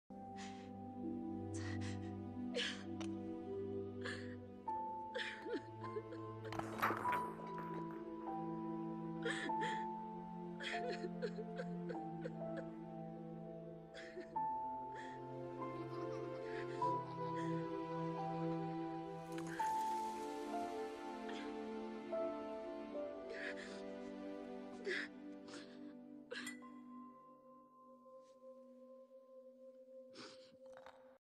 My reaction to when Angela Raiola (a.k.a Big Ang) passed away at the age of 55
If you don't know who she is, she's an actress. She was on the TV show "Mob Wives" on VH1. She also had her own show on VH1 called "Big Ang". She also went to my school sites dance. Big Ang, if you are watching this in heaven, I just wanted to say that you were a really great actress & everyone will miss you so much.